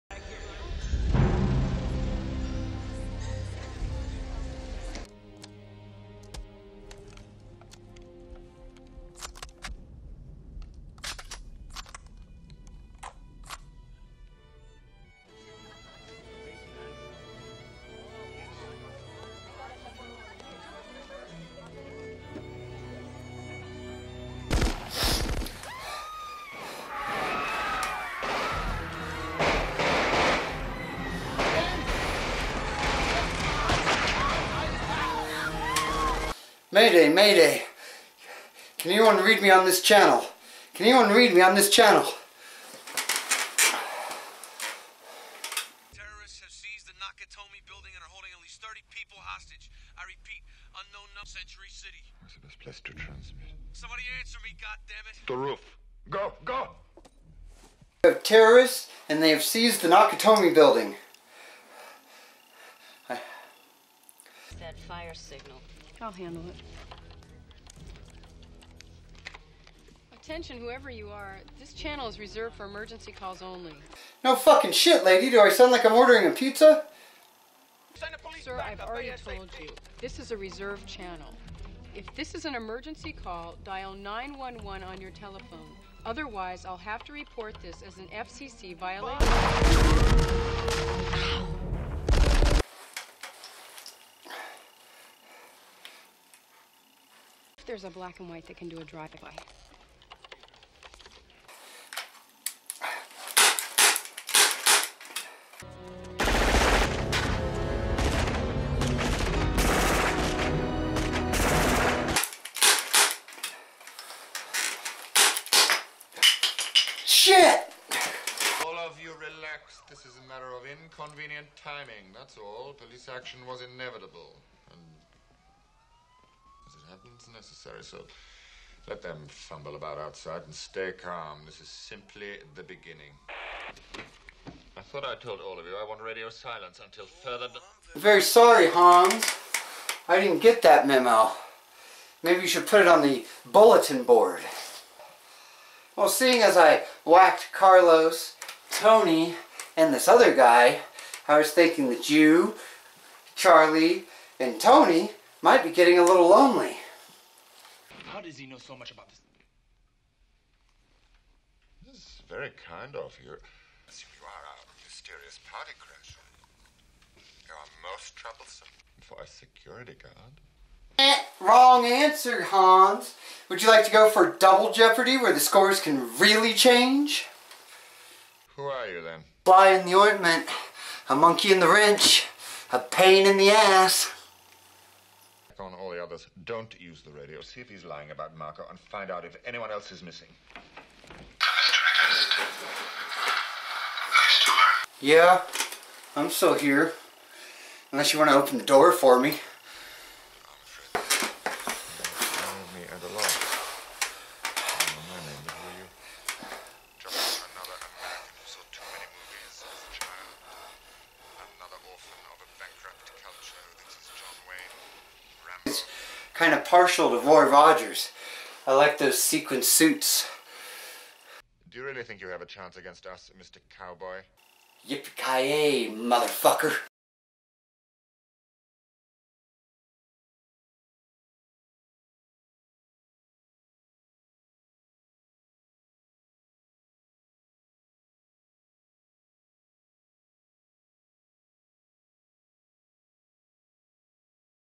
Die Hard Parody
Die Hard
A parody of Die Hard. With someone other then Bruce Willis playing John Mcclain
To buy Die hard at best price